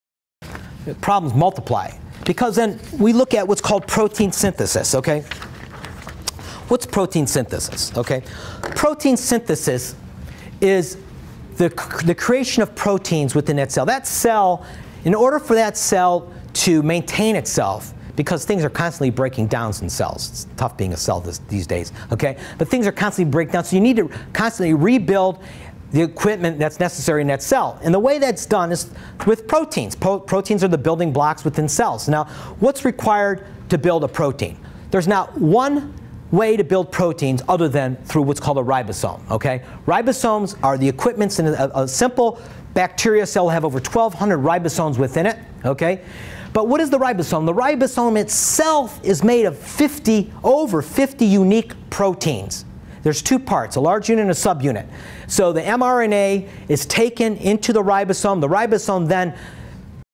Protein Synthesis: Proof of God and the Myth of Evolution Revealed
The next time you eat steak, fish or a protein shake, ask yourself the question, how are proteins made.  Proteins are the result of genetic codes within DNA.  For example, Keratin is a protein that makes up nails, hair, feathers and horns.
How are proteins made?  They are made by a “Cellular Machine” called a Ribosome, this machine takes the genetic code (MRNA) and from these codes it assembles unique proteins such as Keratin. Proteins are made from 20 unique amino acids which are assembled to make proteins.
There is just one problem with evolution, the only way you can make proteins is through the Ribosome. Here is the problem, the Ribosome machine is made of over 50 unique proteins.   The machinery to make proteins requires a machine made of proteins.   Without the Ribosome, you have no proteins, without proteins you have no life.
1. Proteins only come from the Ribosome.
2. The Ribosome is made of Proteins.
3. Without an “Intelligent Creator” you cannot have Protein.
4. Therefore, God has to exist for life to exist.